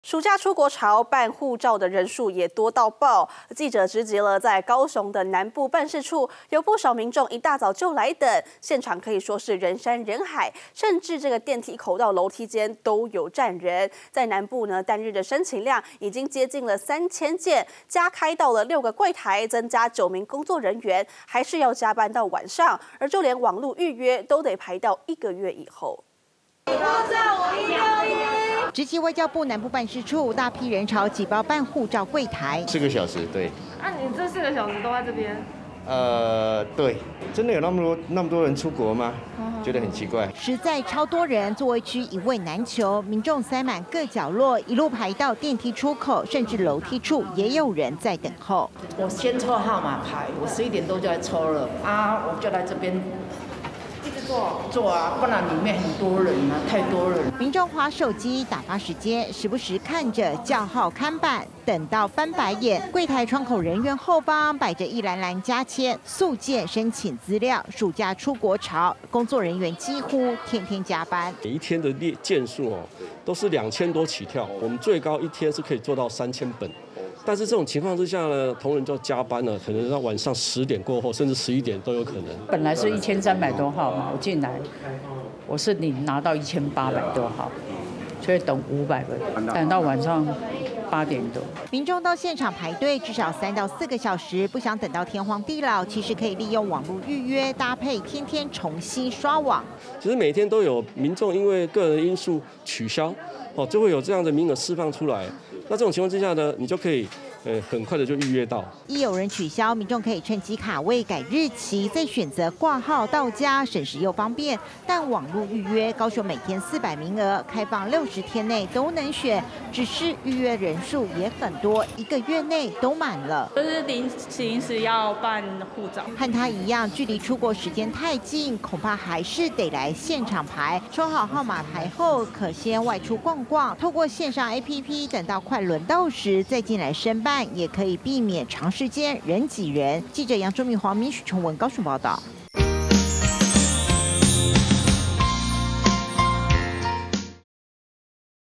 暑假出國潮！ 直擊搶辦護照南部辦事處「塞爆」
暑假出國潮，辦護照人數多到爆，在高雄的南部辦事處，不少民眾一大早就來等。現場人山人海，甚至電梯口和樓梯間都有人站。在南部單日申辦量近3000件，已經加開到6個櫃台、增加9名工作人員，還是要加班到晚上，連網路預約都排到一個月後。